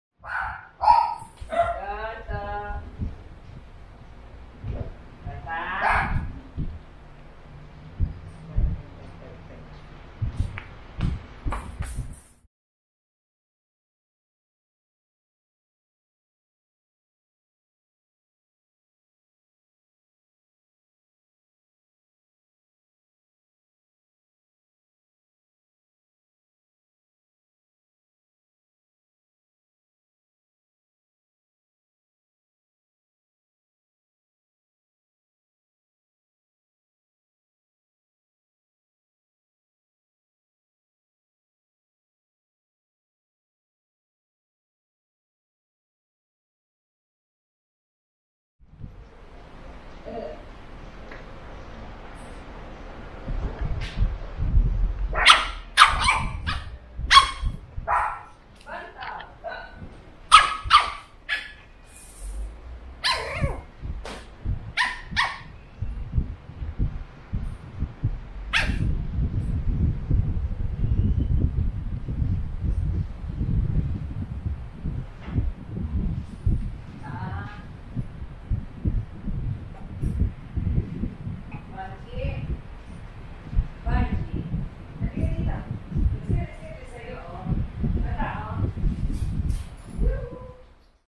Puglet goes crazy in his pen
Burrito, a 13-week old male Pug, plays by himself inside his pen.